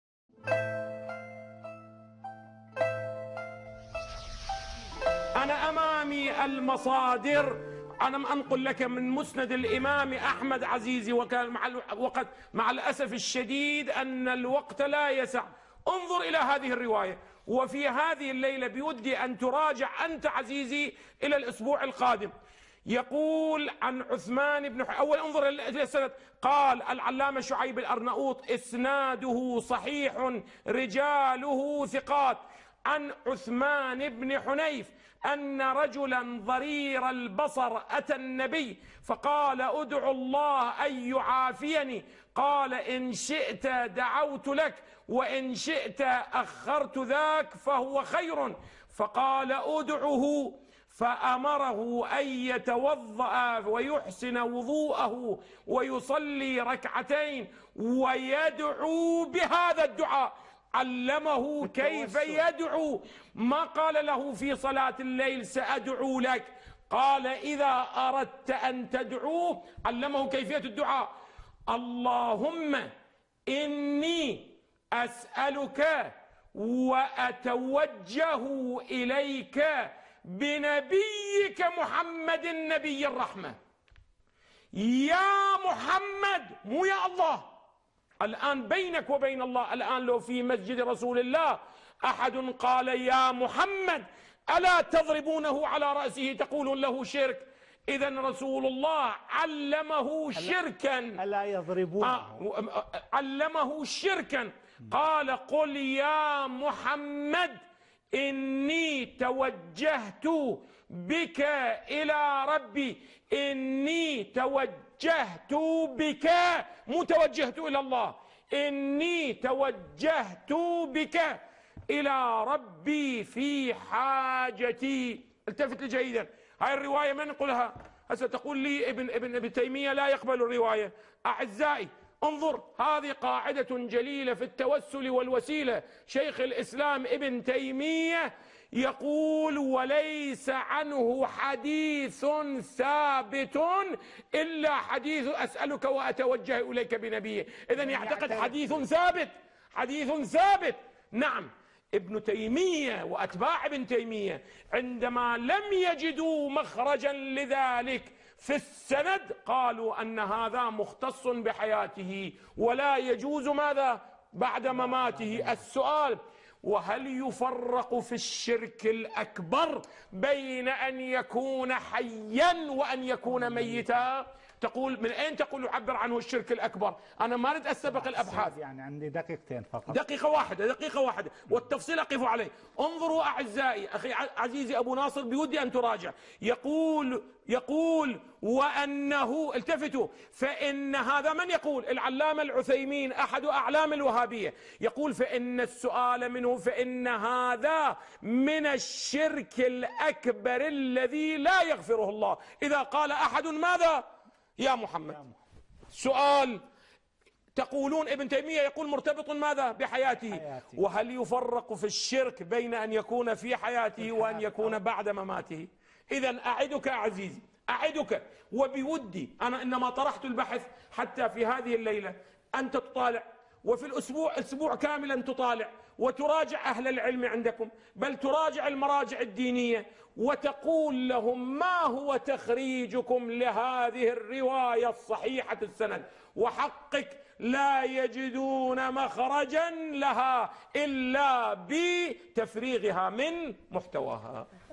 O, Muhammede ! (šiije, šiiti, šije, šiizam)
Sejid Kemal Hajderi: Danas, tako mi Boga, ako bi neko u Poslanikovoj džamiji [u Medini] rekao 'O, Muhammede!' dobio bi po glavi i bio optužen za širk!